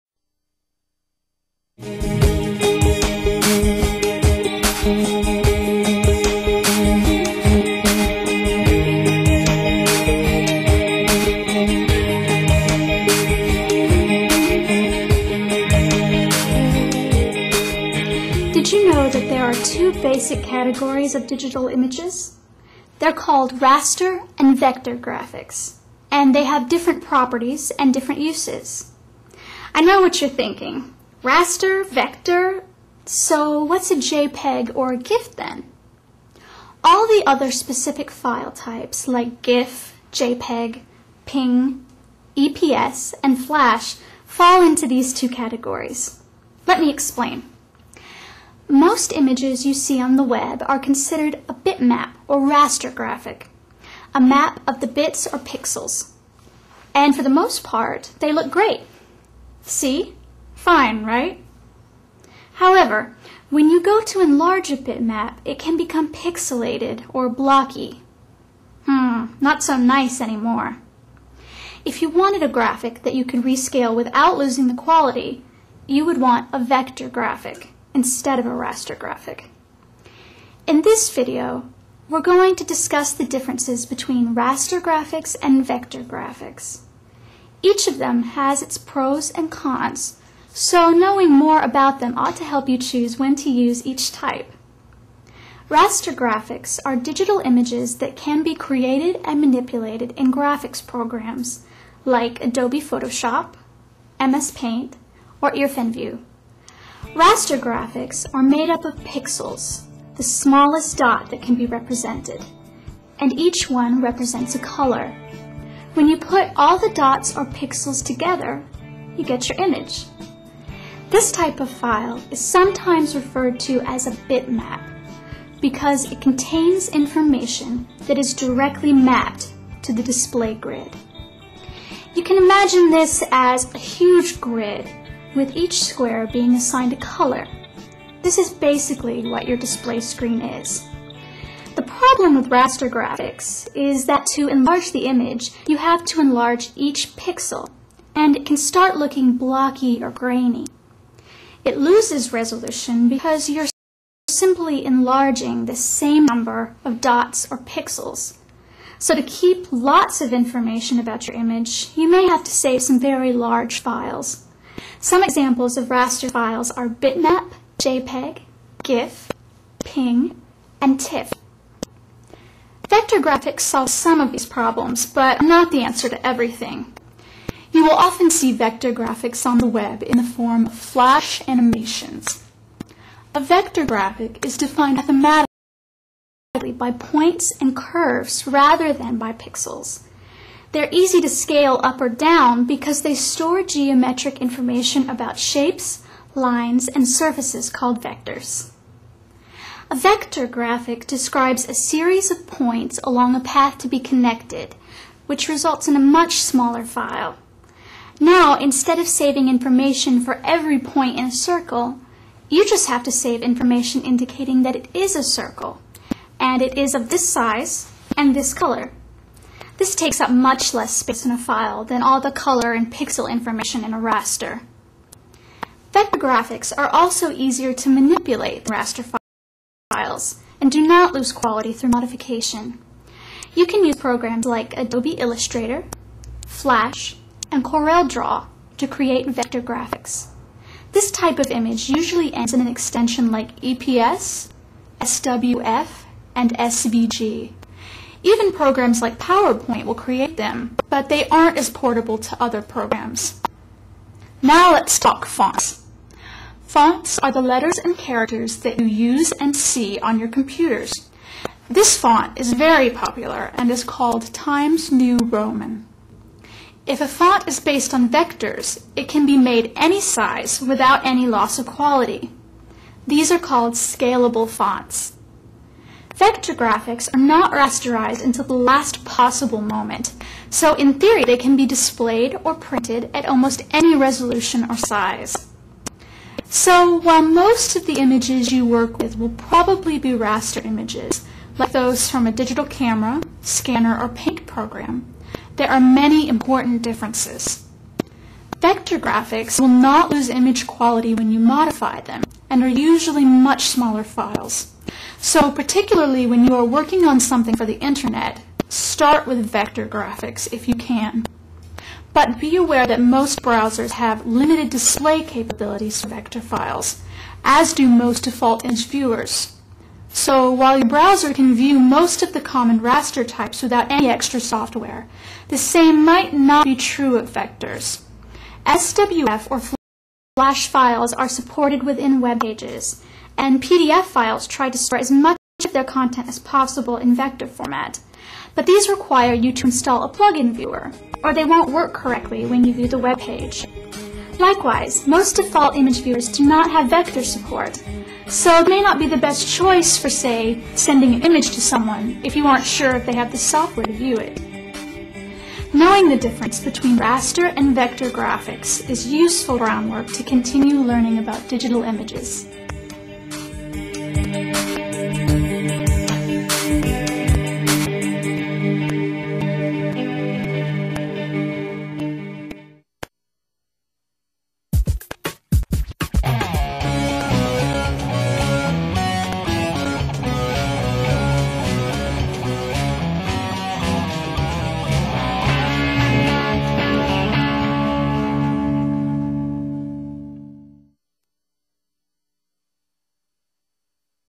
Imaging: Raster and Vector Images
Episode 11 (of a 25-part series) This LearniT tutorial introduces and explains Raster and Vector Images.

LearniT-TeachiT